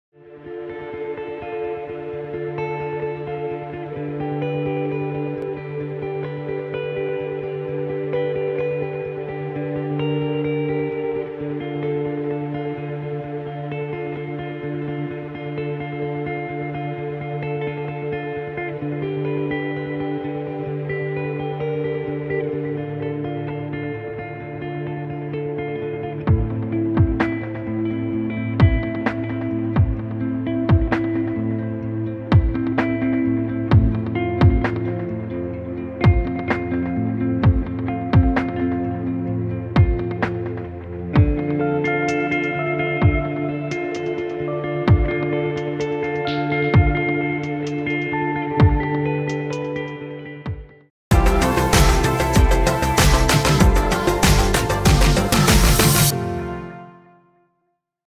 aerial drone real estate video production southeast missouri
This is sample footage of real estate drone video shoot.  To learn more about how you can get UAV drone video footage or pictures of your real estate go